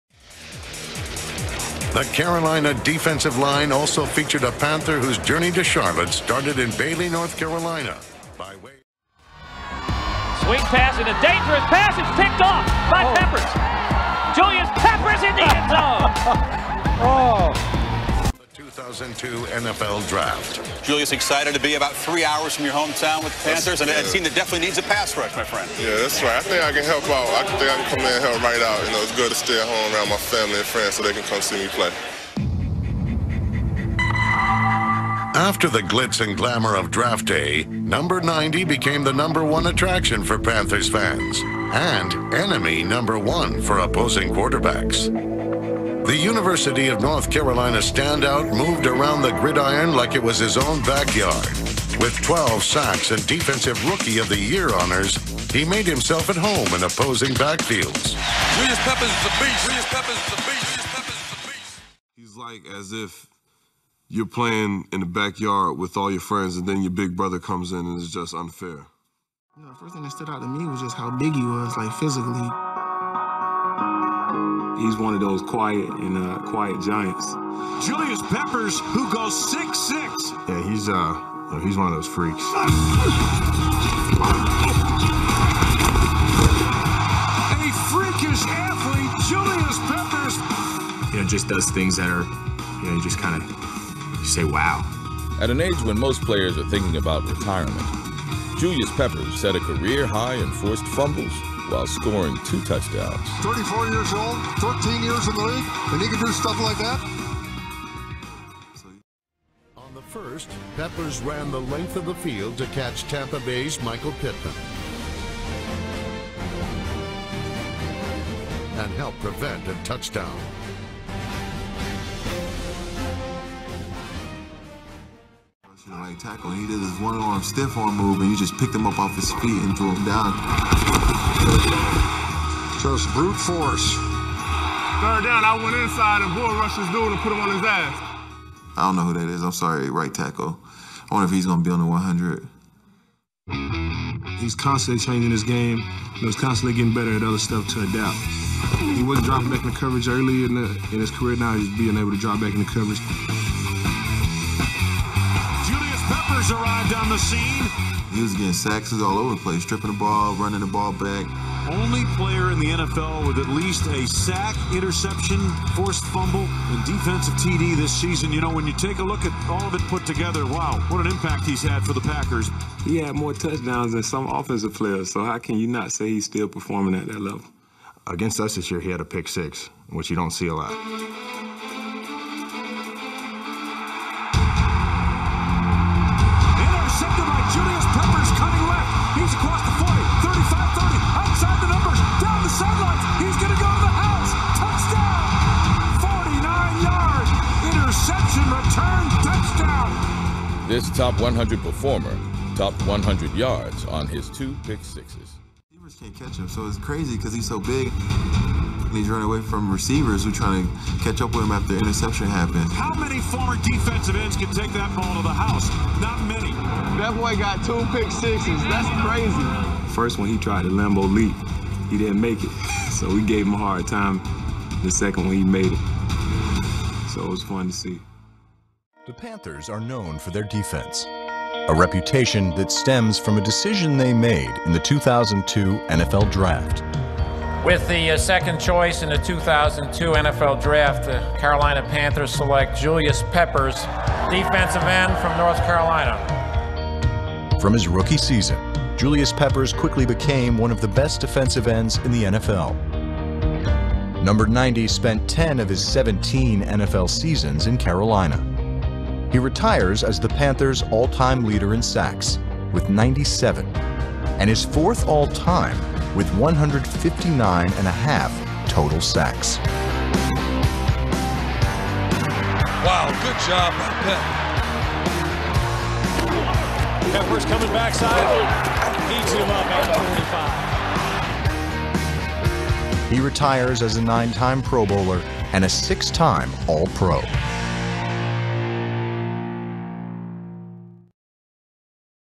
Julius Peppers Highlights
A physical freak (6'7", 295 lbs) and one of the greatest defensive lineman in NFL history. Peppers was drafted by the Panthers in 2002 who went 1-15 the year before and helped them reach the Super Bowl 2 years later. He used his athletic gifts to rush the passer and intercept passes; Peppers tallied 166 total sacks and intercepted 12 total passes in his career. Peppers is a 9x Pro Bowler, 3x 1st Team All Pro, 4th all time in sacks, and 3rd in games played at the defensive end position.